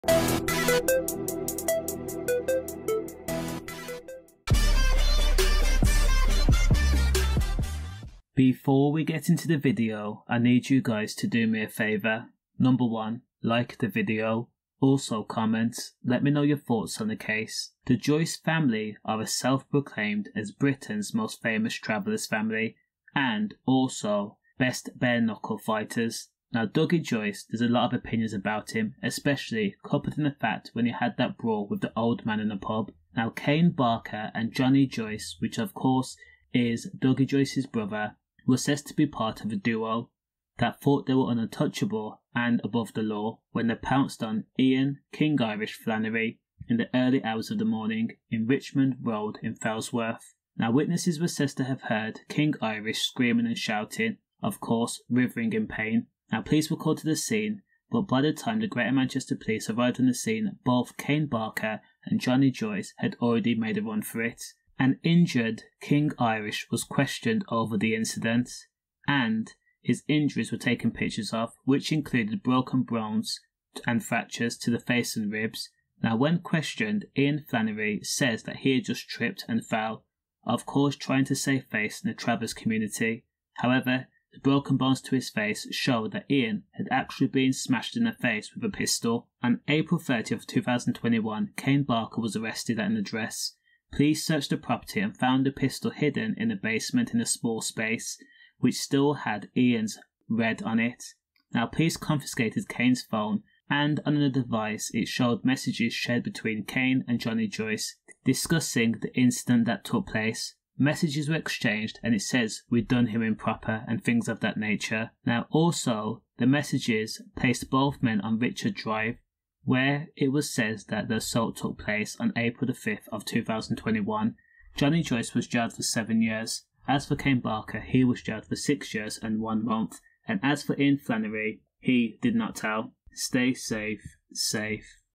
How Travellers Kane Barker and Johnny Joyce were jailed for Att*cking Ian Flannery
kane barker and Johnny Joyce who is part of the famous Joyce traveller family along with Dougie Joyce
was jailed for 7 years after they went to Richmond road in search of Ian king Irish Flannery and then used a pistol to target Ians face and body.
Ian Flannery had broken ribs and broken bones in the face.

police were called to the scene. Ian Flannery told police he fell .

kane barker was found at an address and a g*n was recovered which had Ians red on it. messages showed Johnny Joyce was involved